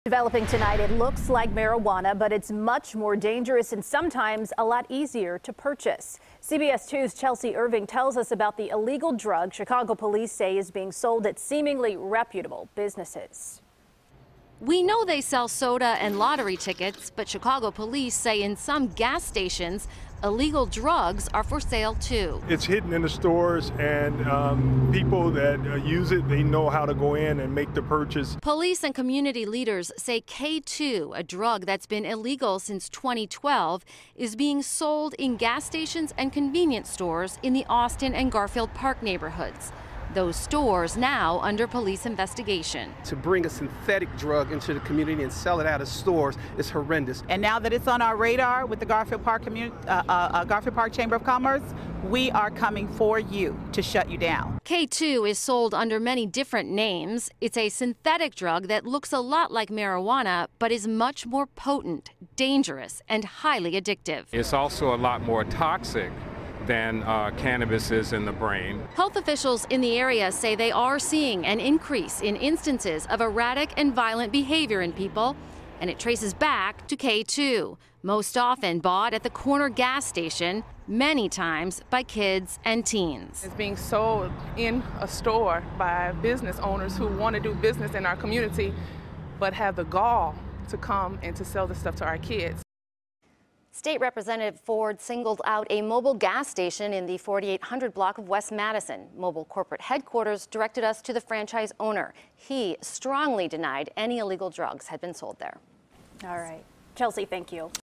Officials Push To Get Synthetic Drug Out Of Stores
A West Side lawmakers is joining others to call for some merchants to stop selling the dangerous synthetic drug "K2." CBS 2's Chelsea Irving reports.